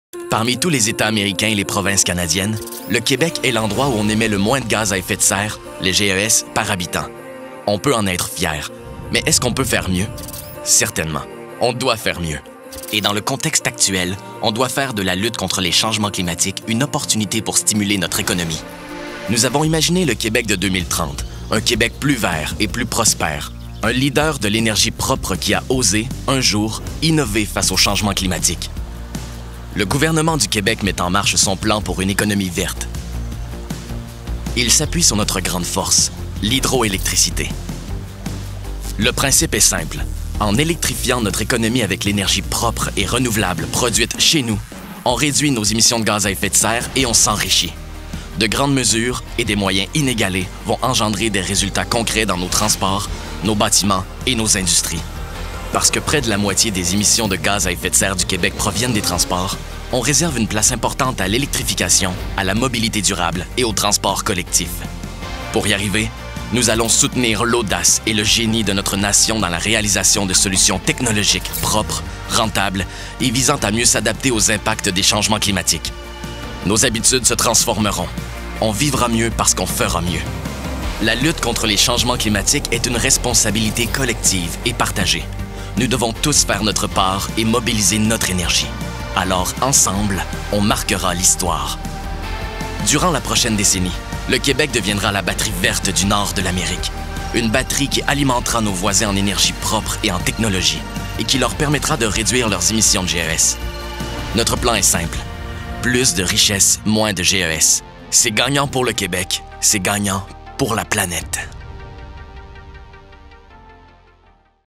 Présentation du Plan pour une économie verte 2030
Le Plan pour une économie verte 2030 est la politique-cadre d’électrification et de lutte contre les changements climatiques qui engage le Québec dans un projet ambitieux jetant les bases d’une économie verte, résiliente face aux changements climatiques et plus prospère à l’horizon 2030.